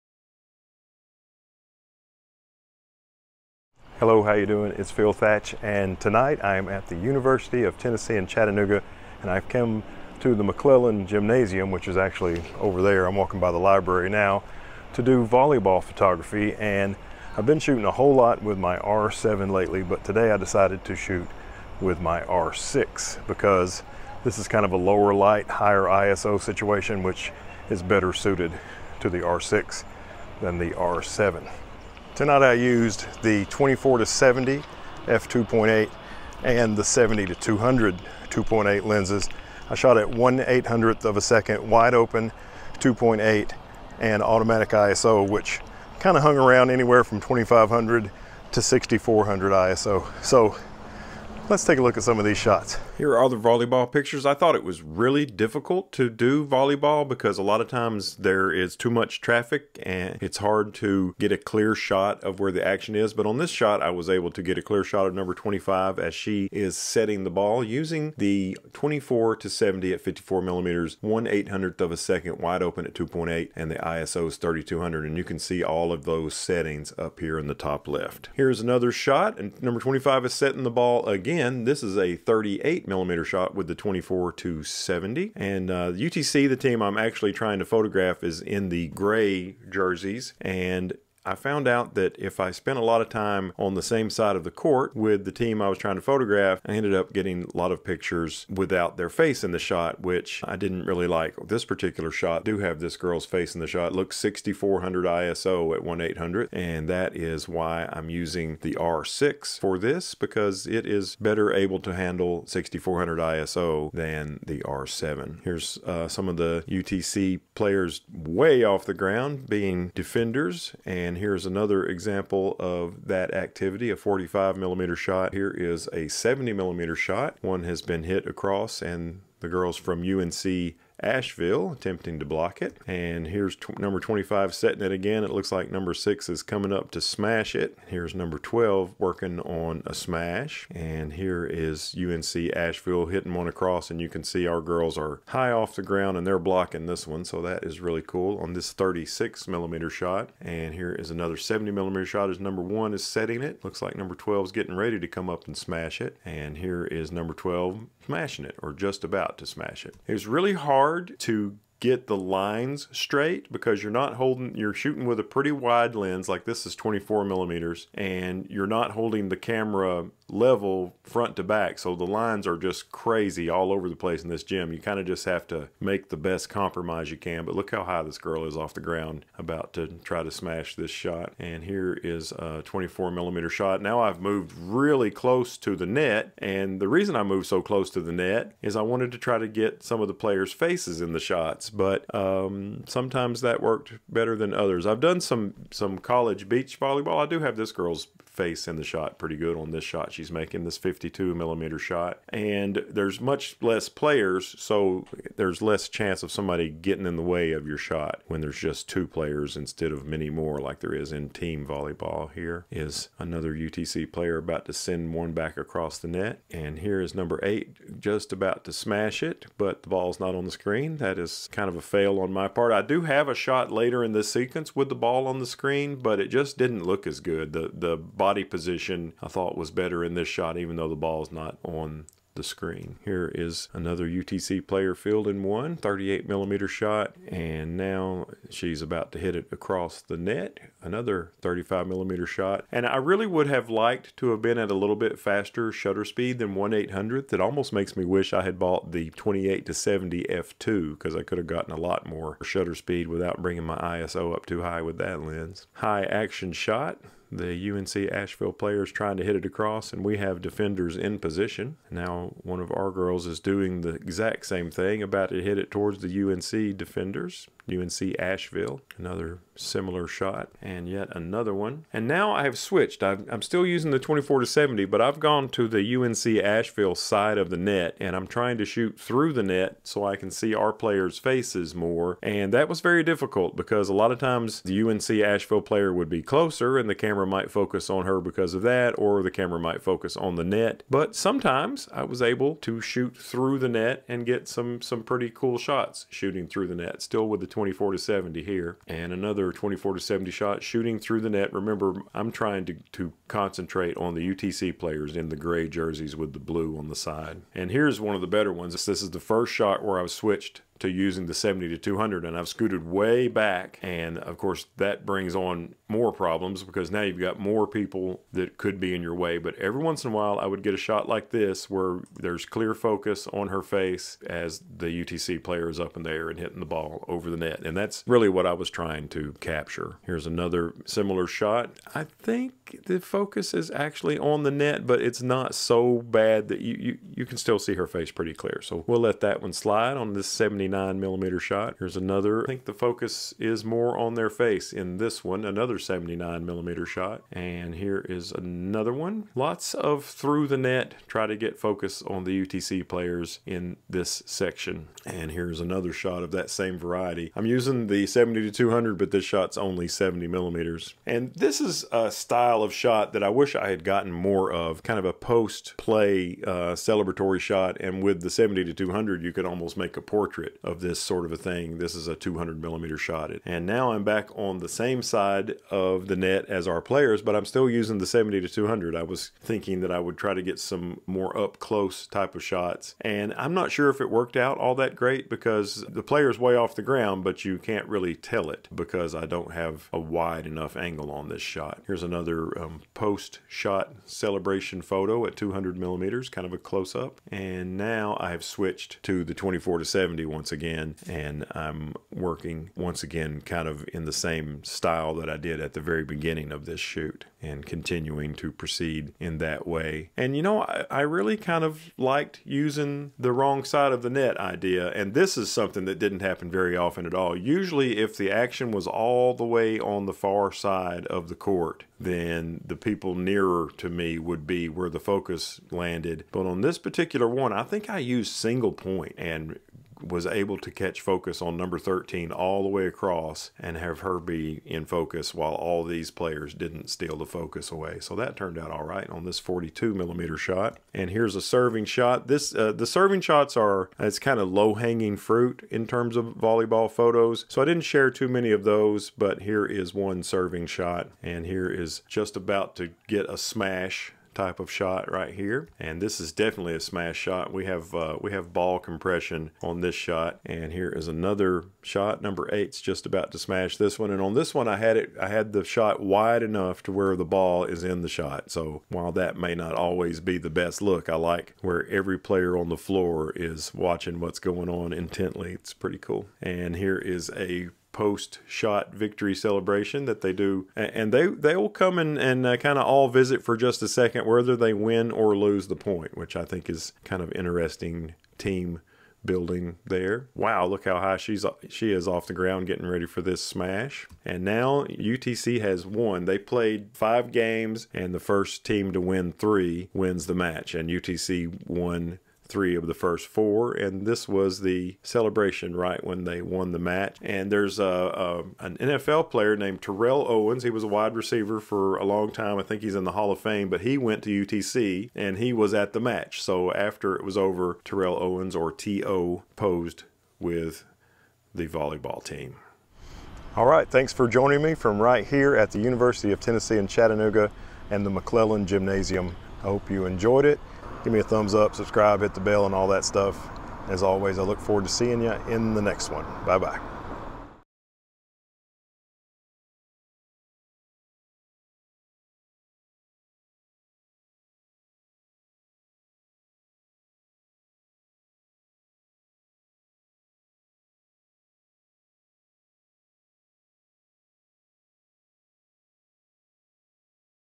Canon R6 College Volleyball Photography
Using my Canon EOS R6 and the RF 24-70 and RF 70-200 lenses I photograph college soccer at the University of Tennessee at Chattanooga. I found it to be a difficult sport to photograph but still fun.

Look for me on Vero @philthach
Heather is on Vero too @heatherbrie